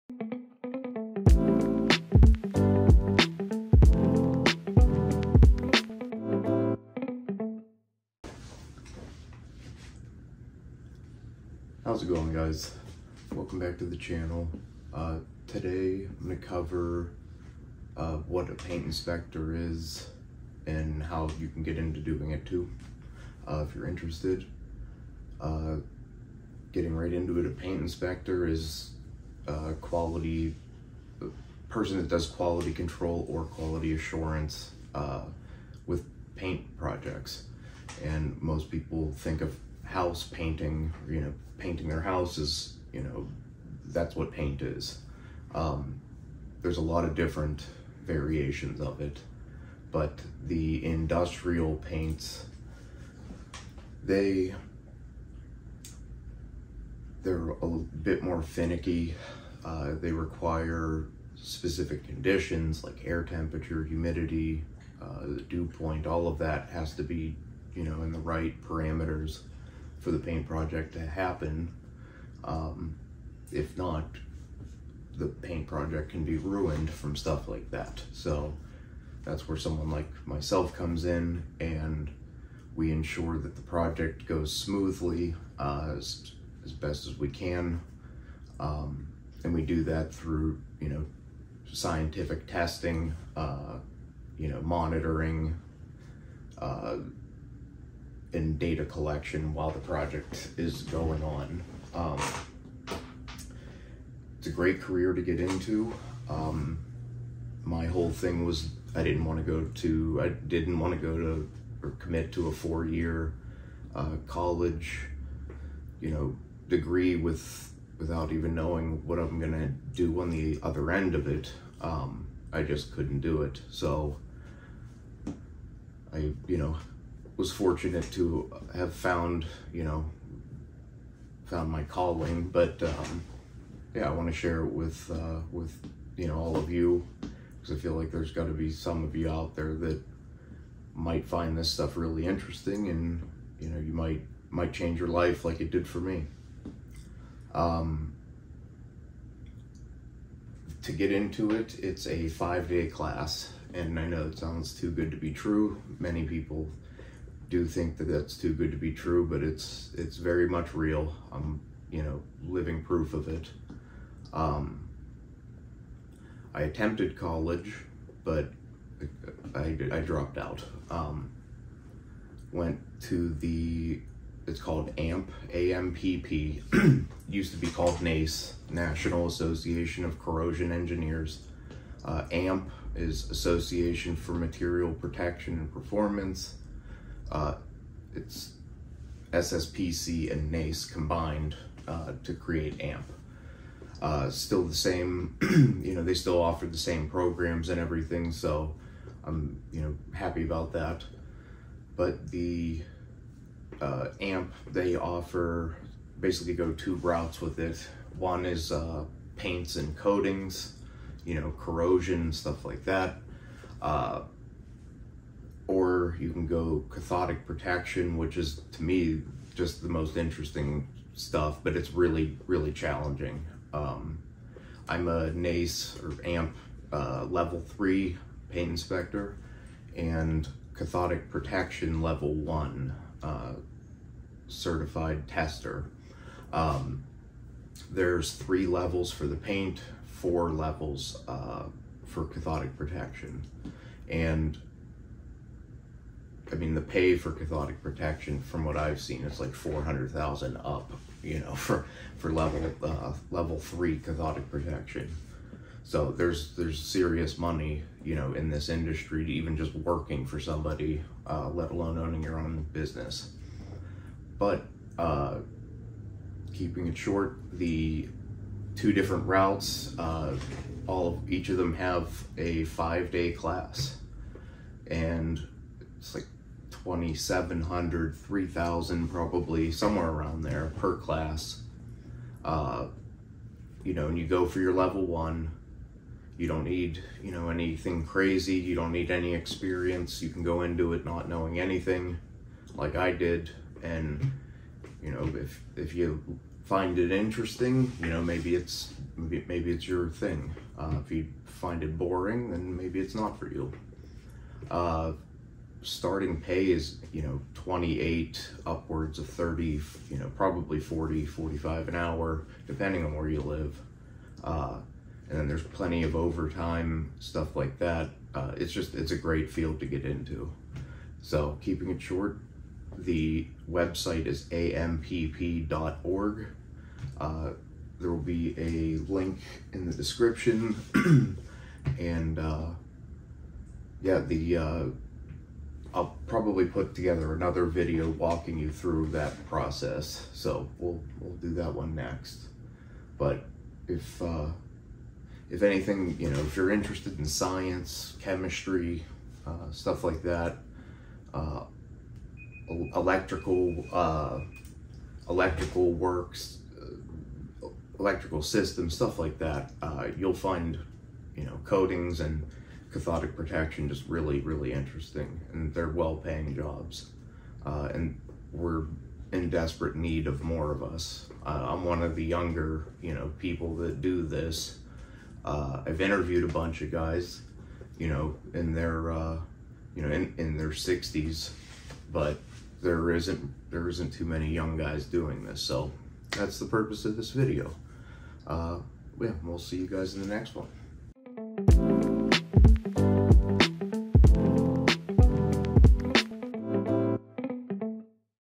How to become a Paint Inspector through AMPP
Sharing a brief story on what an AMPP Paint Inspector is, and how to become one. AMPP =  Association for Material Protection & Performance

Global association that manages the education and standards for corrosion, paints, lining systems, and even fiberglass / carbon fiber.

To get your foot in the door of a cool industry, all it takes is a 5 - 6 day class, and $3,000  USD, and you're set for life. AMPP Basic Coating Inspector Level 1.